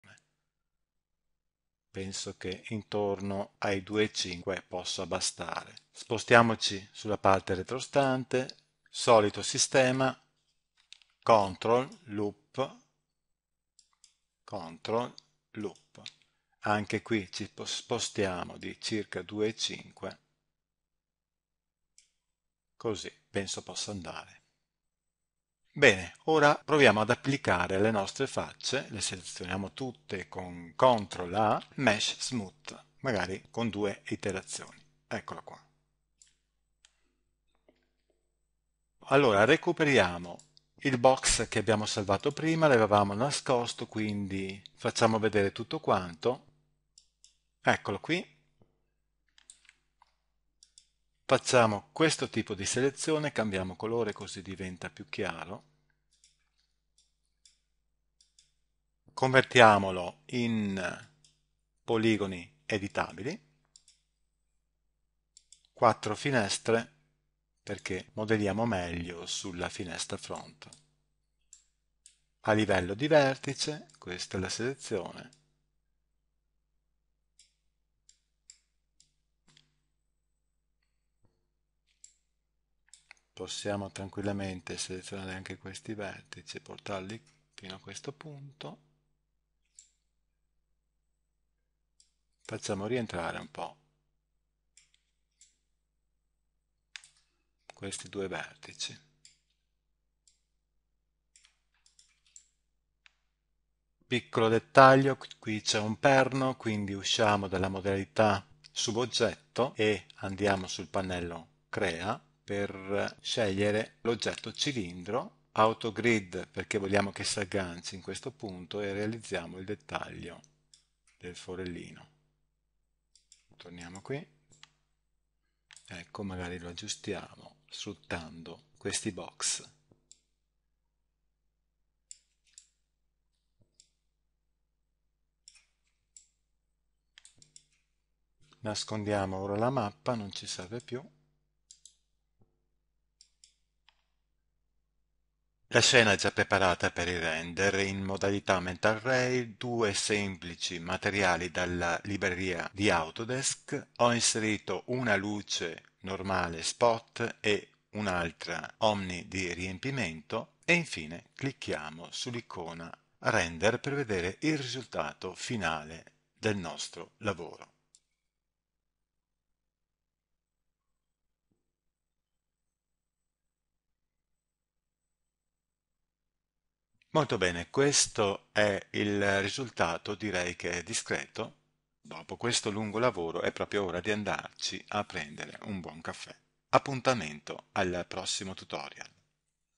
Box Modeling - Modellare una moka da caffè in 3Ds Max - part 4
Questo video ha una durata limitata perché è in versione DEMO. La versione integrale la trovate all'interno di "3Ds Max - La grande video guida"
Video tutorial in quattro Lezioni su come modellare una Moka con la tecnica Box Modeling in 3Ds Max (Manico e Render finale) - Part 4. Diventa un Professionista di AutoCAD e 3Ds Max con i video corsi su